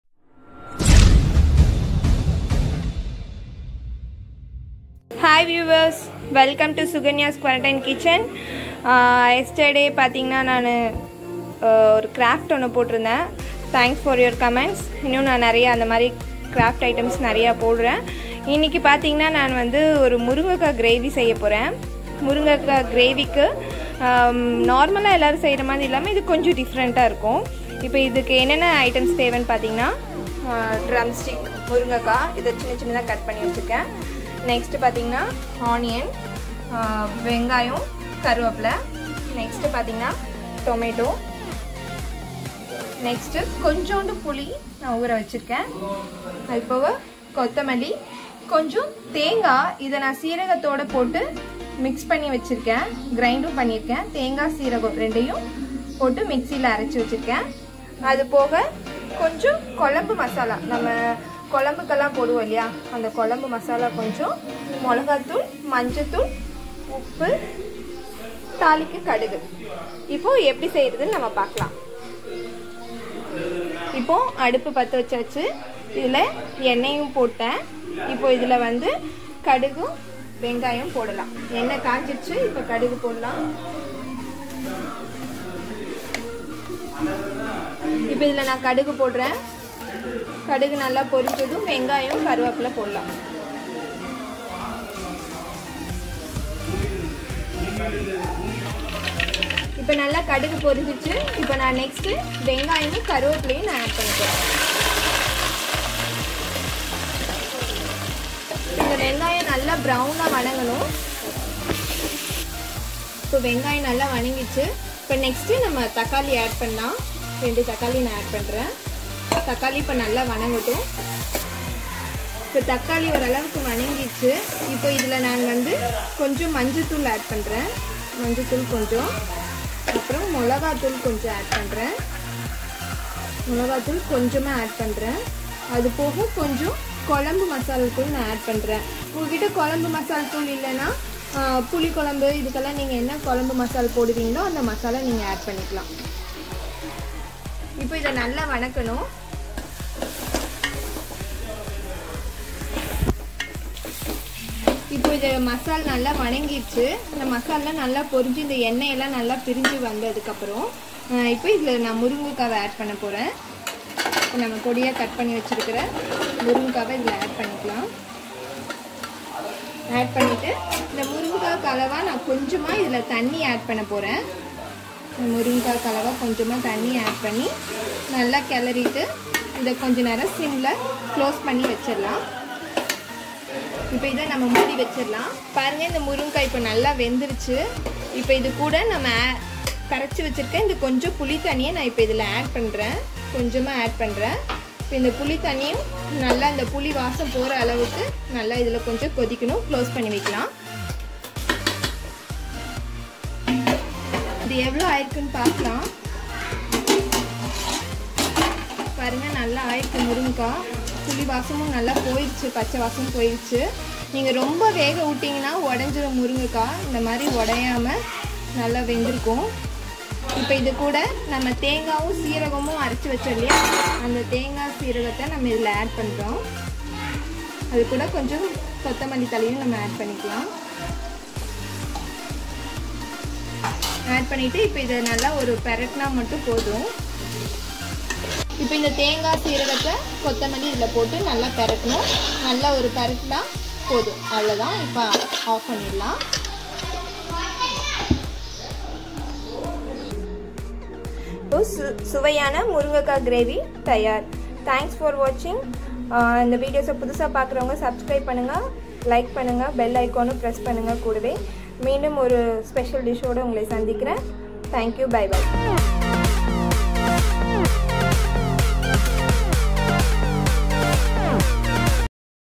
இப்படியும் முருங்கைக்காய் செய்யலாம் பாருங்க......How to make Drumstick curry.......
Ingredients
1. Drumstick
2. Onion
3. Tomato
4. Tamarind
5. Coconut
6. Curry leaves
7. Coriander
8. Cumin
9. Chilly powder
10. Turmeric powder
11. Mustard seed
12. Salt
13. Oil